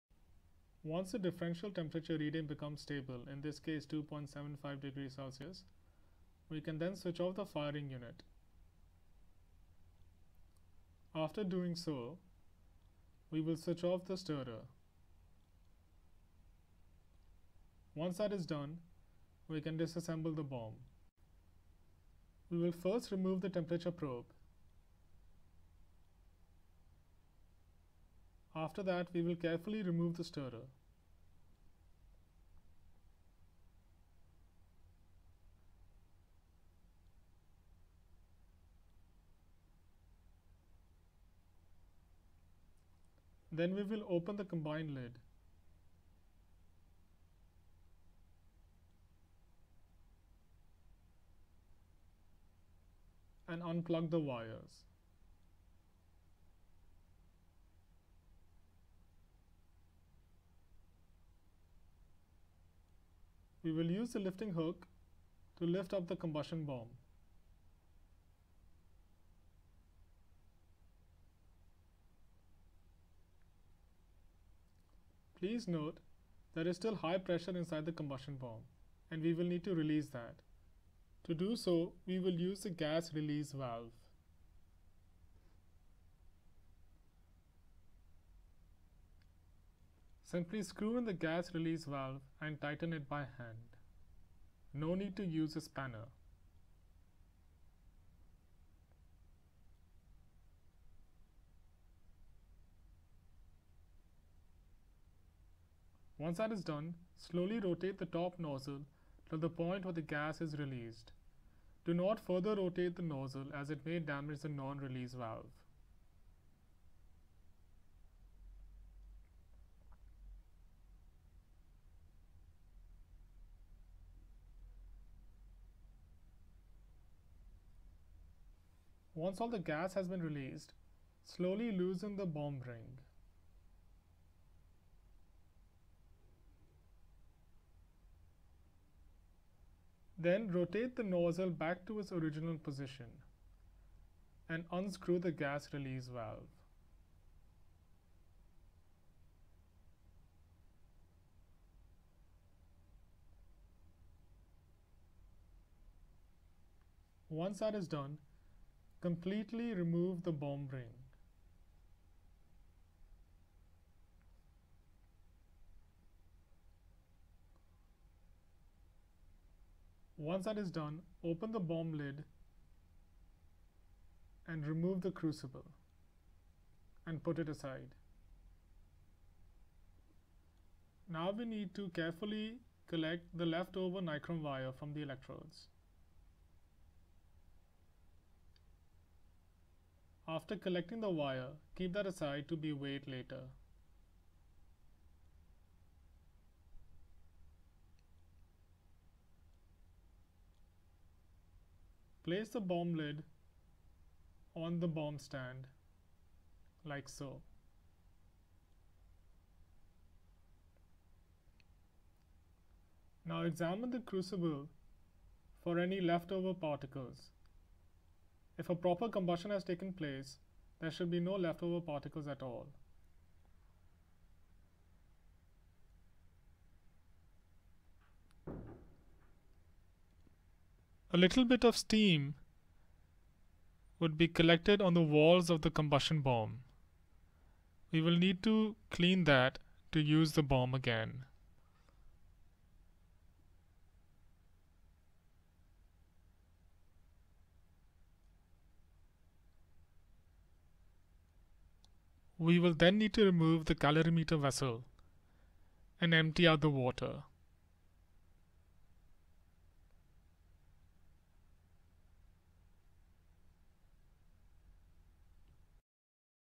Toshniwal Calorimeter Part 6/6 - Disassembling the Calorimeter Instrument
Toshniwal Technologies Calorimeters are the most reliable, value for money Calorimeters in the market. This video series explains the installation process of the Calorimeter. Part 6 of the video explains how to disassemble the entire Calorimeter Instrument.